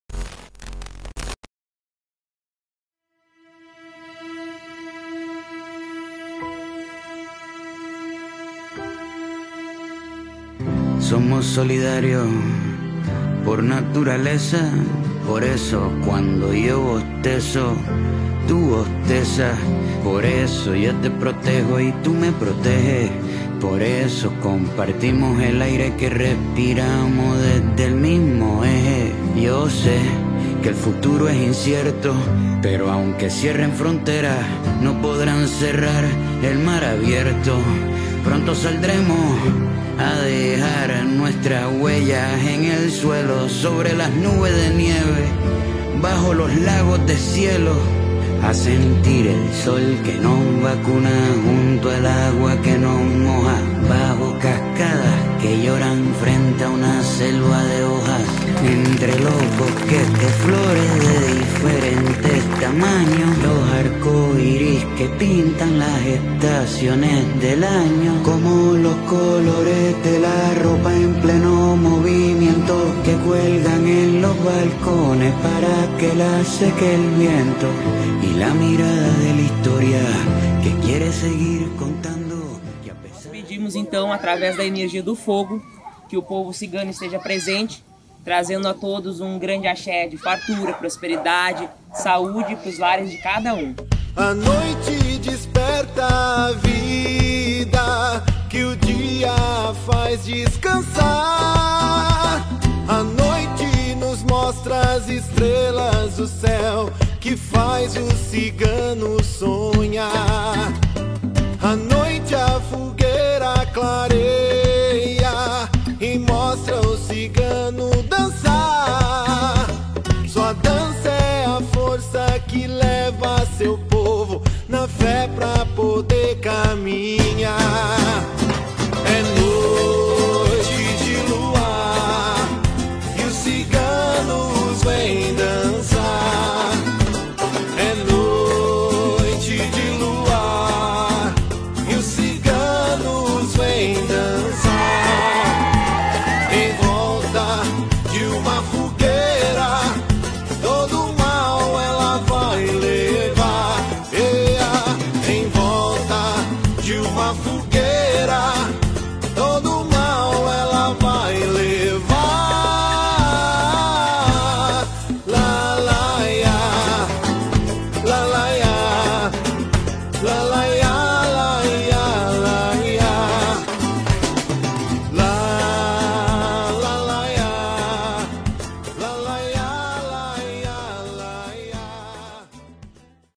Lydia & Beto Casamento Cigano
Casamento Cigano da Lydia & Beto celebrando amor e a união